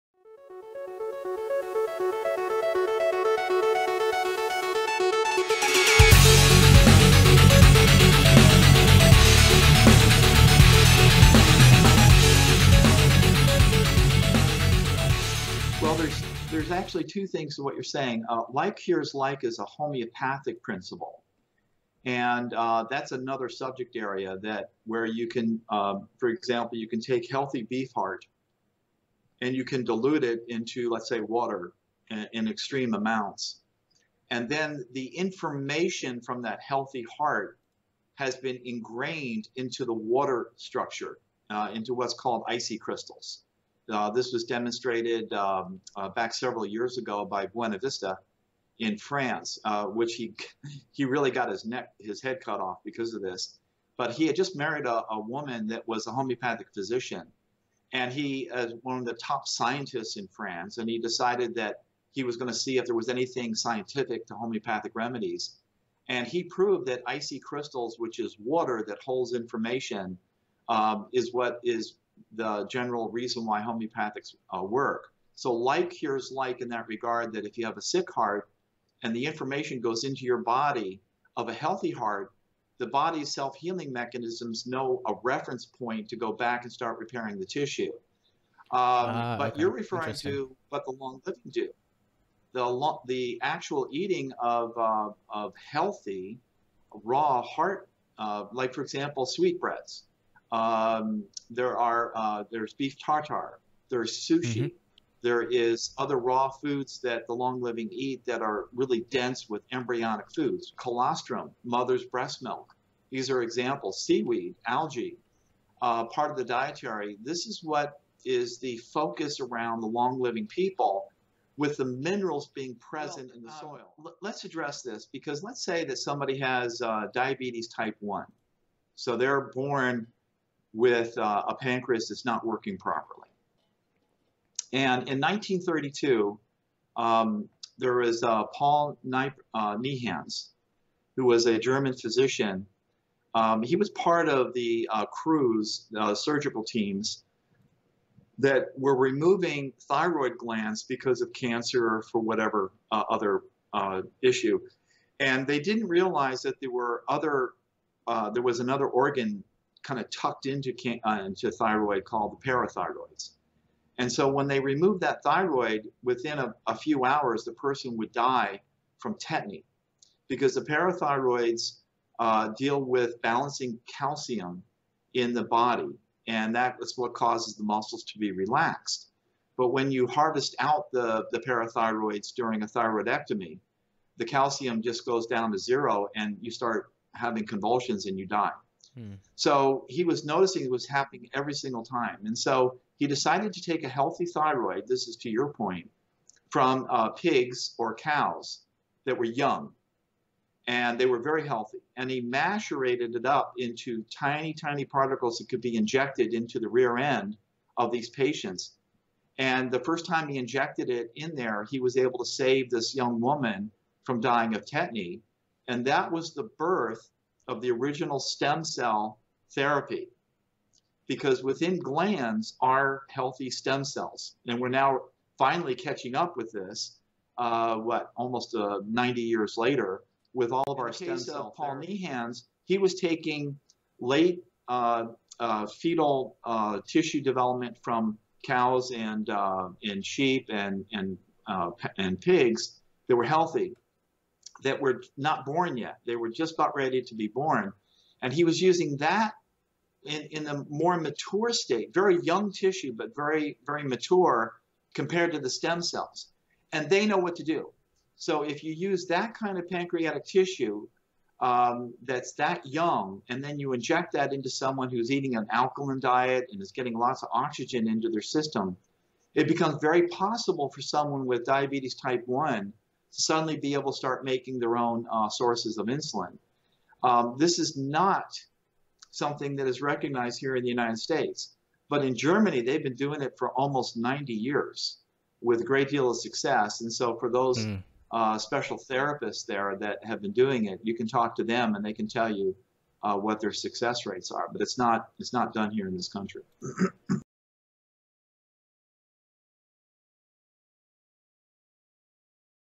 ["Like Cures Like"] Using Animal Organs/Stem Cells To Regenerate The Body
Video excerpts from The Vonu Podcast's HealthLiberation / SelfLiberation series, with guest, Dr. John Apsley.

[LIBERTY UNDER ATTACK PUBLICATIONS 2020 SPECIAL]
🏴Get a FREE paperback copy of Kerry Thornley's, "The Permanent Floating Voluntary Society" with an order of any amount!🏴